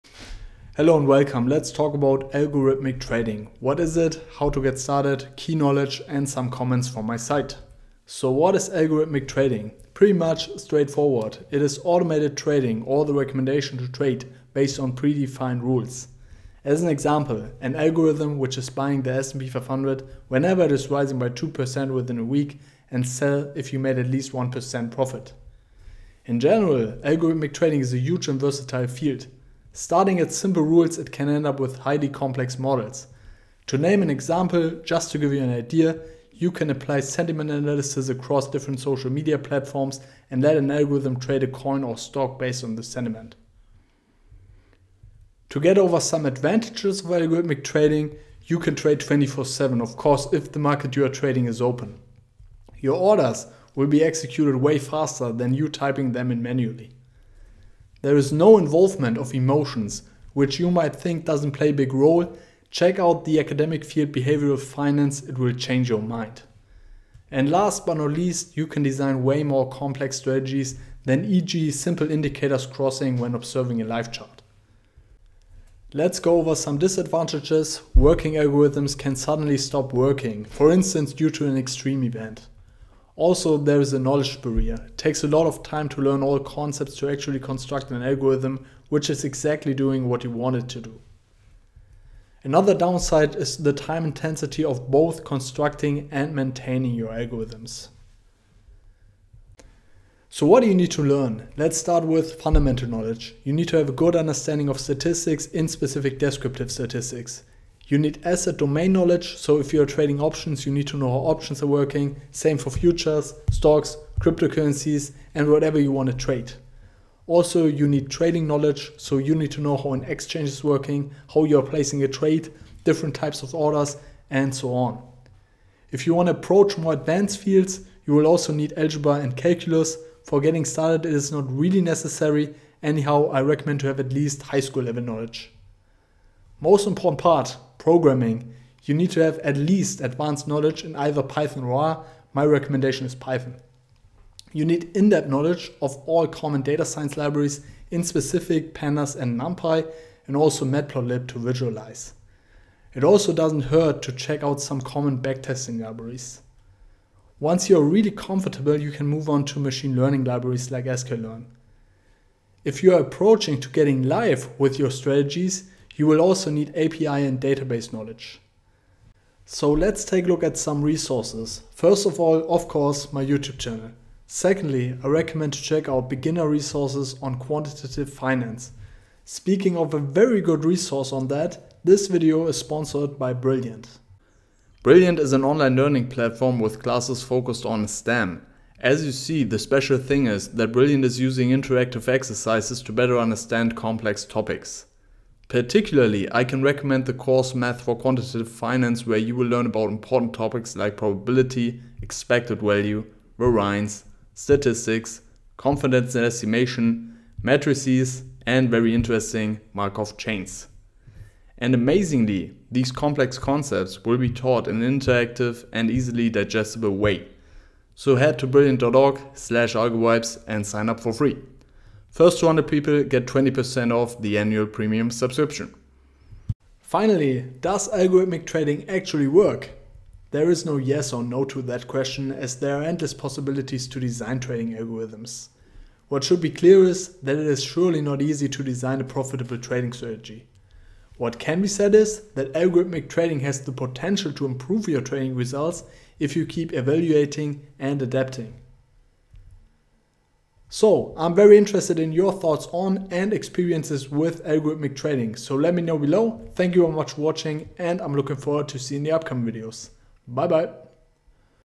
How to get started with Algorithmic Trading?
In this video we are talking about what Algorithmic Trading is, how to get started, some advantages and disadvantages, what do you need to learn and answer some common questions.

Let me know your thoughts and/or experiences in the comments below!

Be invited to check out my 1 hour Python course to get started: